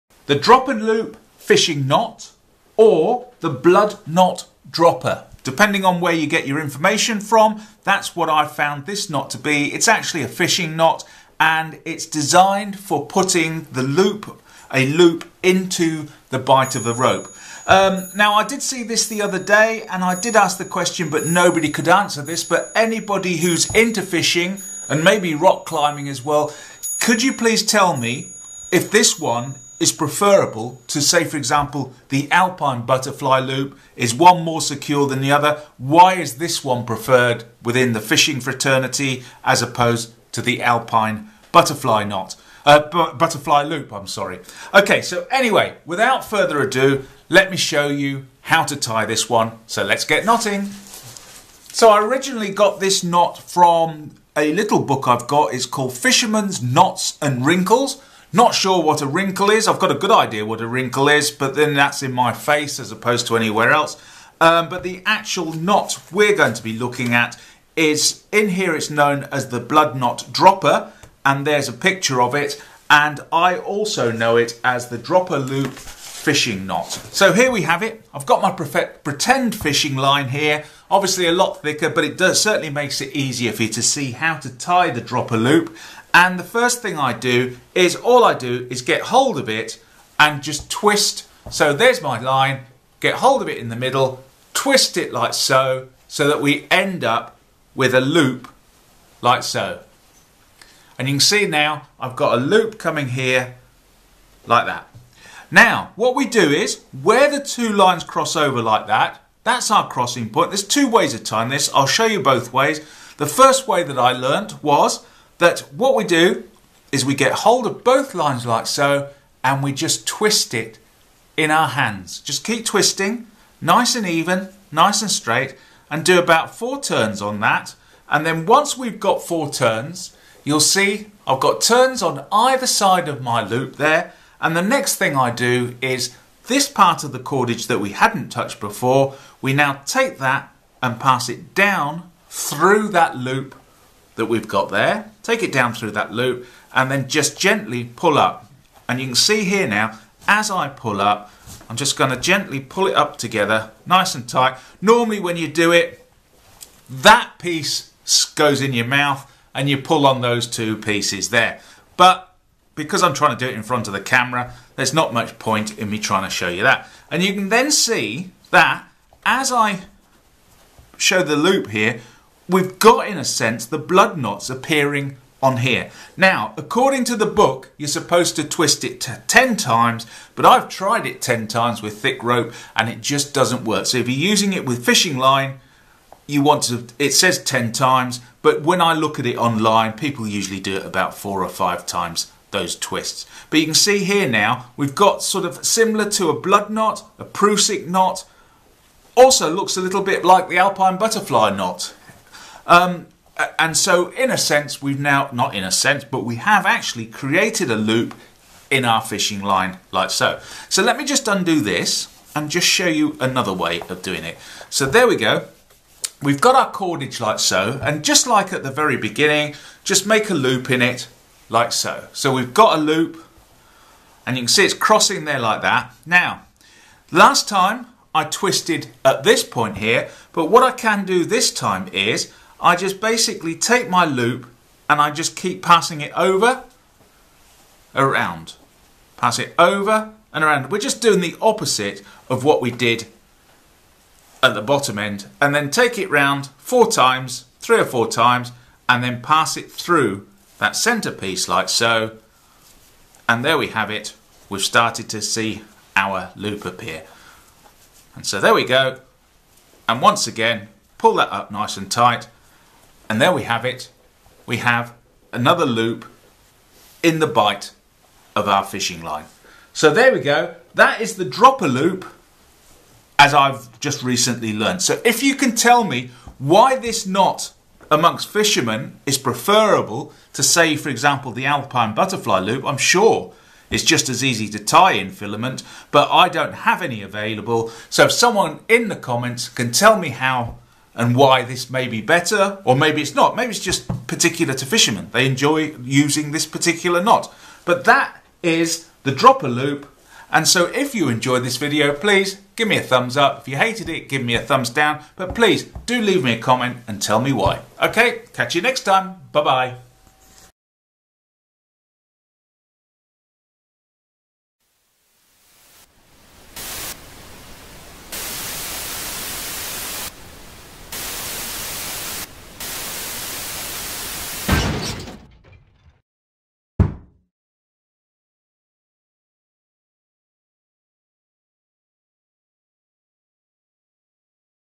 Dropper Loop Fishing Knot or Blood Knot Dropper Making a Loop in the Bight of a (Fishing Line Loop)
Dropper Loop Fishing Knot or Blood Knot Dropper Making a Loop in the Bight of a Fishing Line.
How to Tie the Dropper Loop.

The Blood Knot Dropper is made by first making a circle in the cast, and then twisting one loose end through it 10 times, so that 10 small circles are made. Enlarge the middle circle, and then through it push the top of the original circle. Pull tight, leaving a small loop on which the dropper may be tied.

 Channel Handle: @KnottingKnots